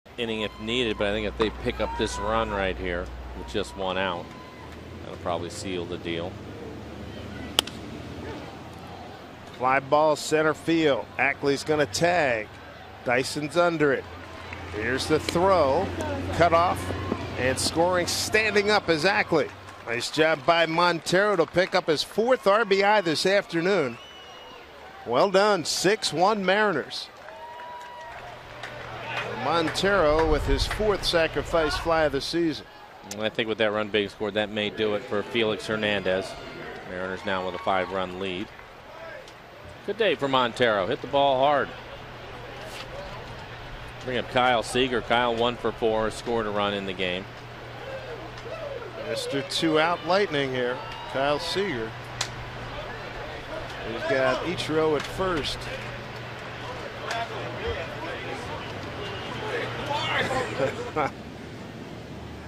2012/07/19 Montero's sacrifice fly
SEA@KC: Montero lifts a sacrifice fly to center